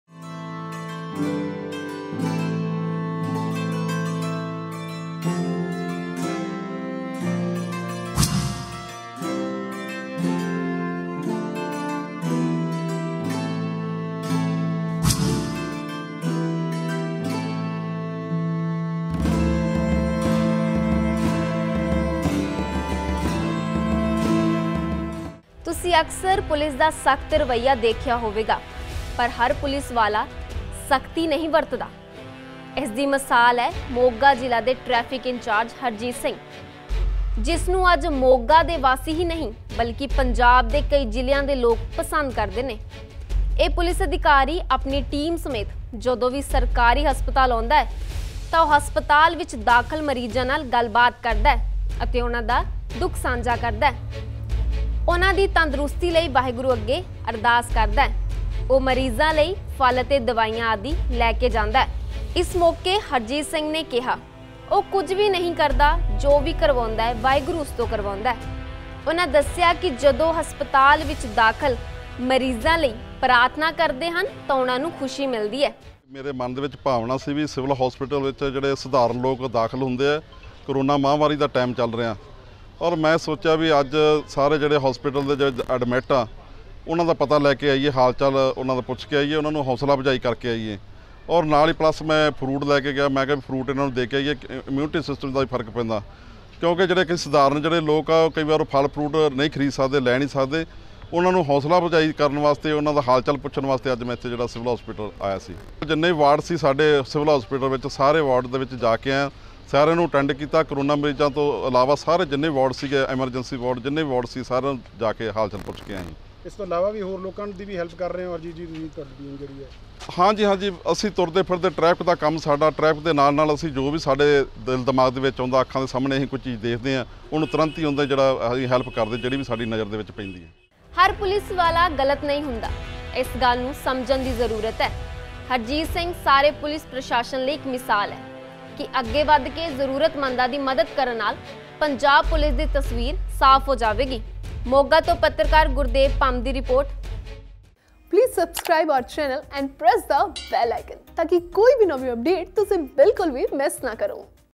ਮੋਗਾ ਦਾ ਟ੍ਰੈਫਿਕ ਇੰਚਾਰਜ ਨੇ ਪੇਸ਼ ਕੀਤੀ ਇੰਸਾਨੀਅਤ ਦੀ ਮਿਸਾਲ | Sanjha TV
ਮੋਗਾ ਦਾ ਟ੍ਰੈਫਿਕ ਇੰਚਾਰਜ ਨੇ ਪੇਸ਼ ਕੀਤੀ ਇੰਸਾਨੀਅਤ ਦੀ ਮਿਸਾਲ
ਹਸਪਤਾਲ 'ਚ ਦਾਖਲ ਮਰੀਜ਼ਾਂ ਦਾ ਦੁੱਖ ਕਰਦਾ ਹੈ ਸਾਂਝਾ
ਫਲ ਅਤੇ ਦਵਾਈਆਂ ਦੇ ਕੇ ਮਰੀਜ਼ਾਂ ਦੀ ਕਰਦਾ ਹੈ ਮਦਦ

24 Hours Local Punjabi Channel
Telus # 2424
VMedia # 801
RokuTV

RokuTV
Watcho-d2hApp
mxplayer